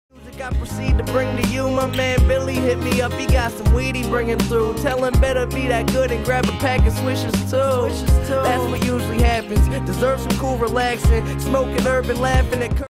Jason Kidd HUGE pass to Richard Jefferson
Jason Kidd gives a tremendous pass to Richard Jefferson playing with the New Jersey Nets.